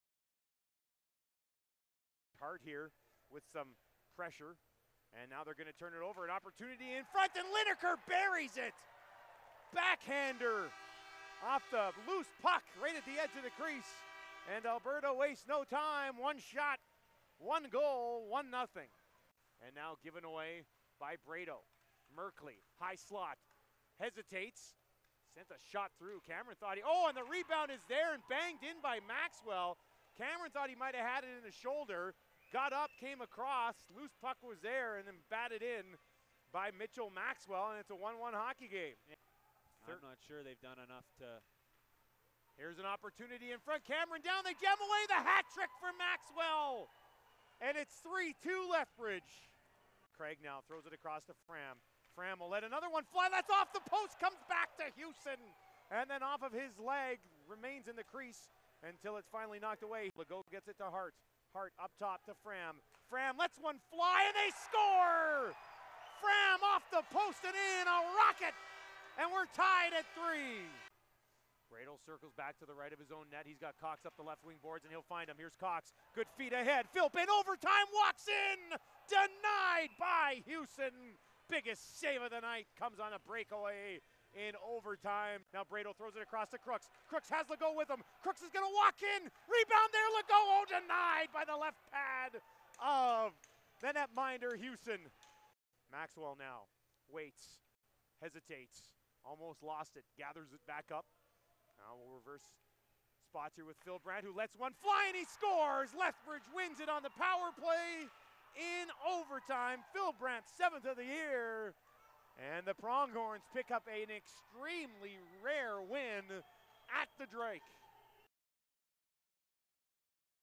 Highlights: Golden Bears Hockey vs Pronghorns - Feb 4
Lethbridge Pronghorns beat the University of Alberta Golden Bears 4-3 in Double OT.